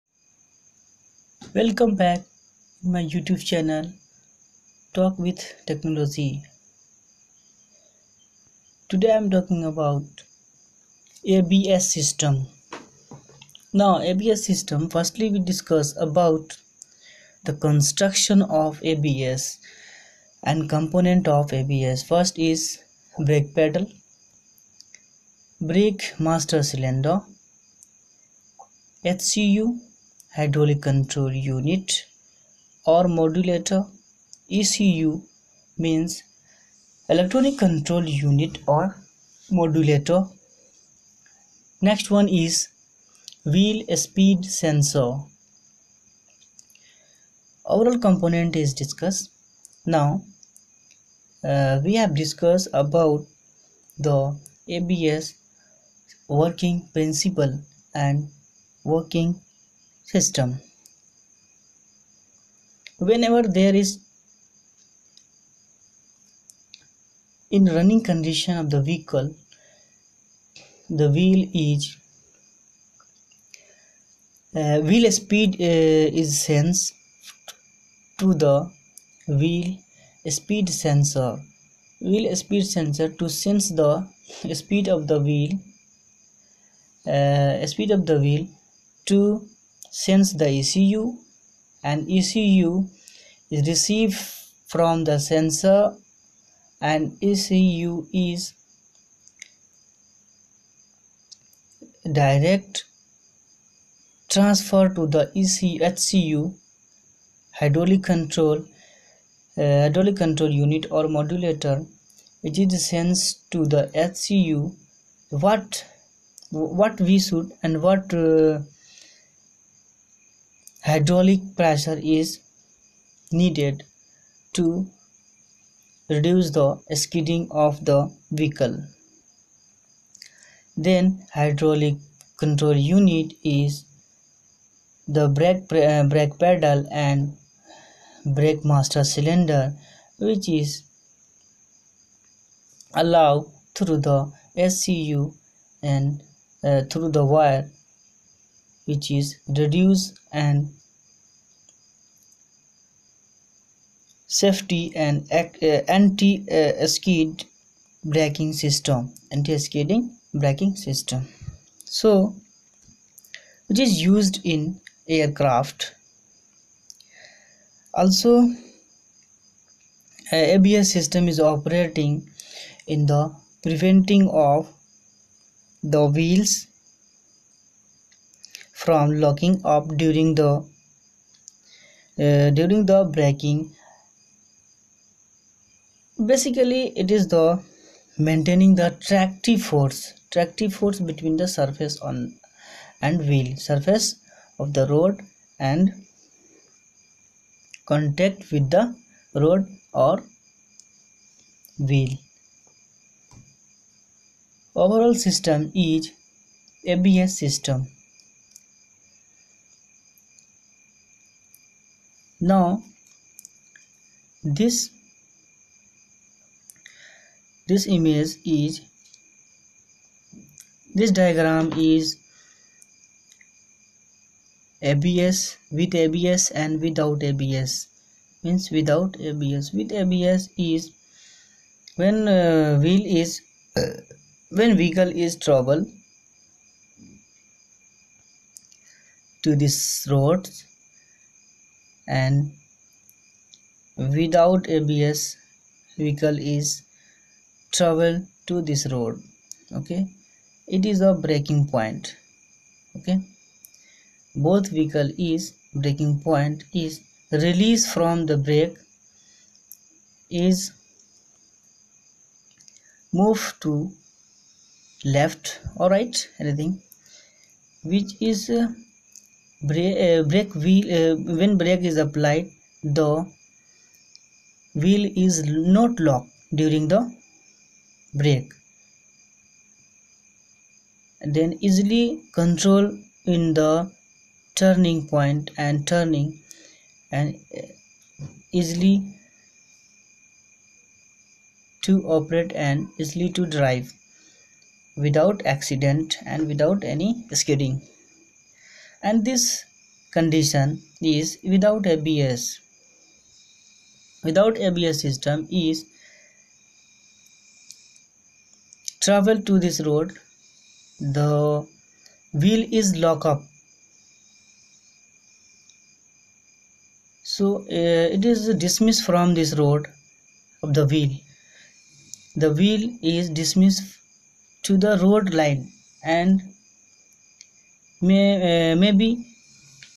ABS system.Antilock braking system in English.
How to work Abs system. Construction of ABS system in English.how to reduce the skidding of wheels.it is basically reduced and safety the anti skid braking system.